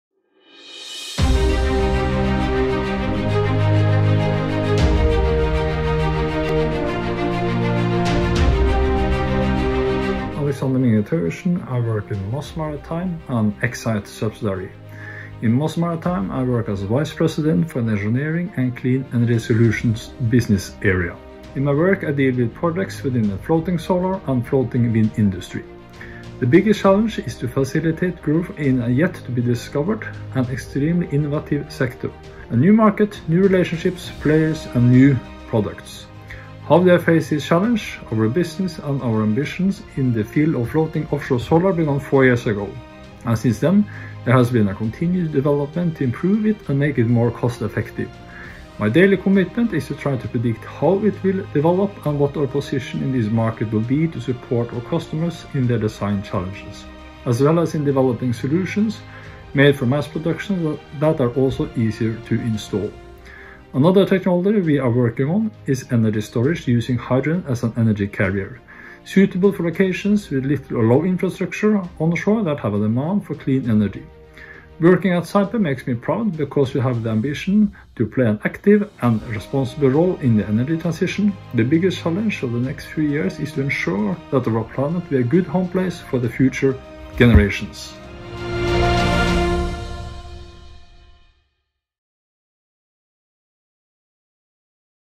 Saipem | Our Story, Alexander Minge Thøgersen, Vice President Engineering and Clean Energy Solutions
In my work I deal with projects within the floating solar and floating wind industry. The biggest challenge is to facilitate growth in a yet to be discovered and extremely innovative sector: a new market, new relationships, players and new products.

How do I face this challenge? Watch the video!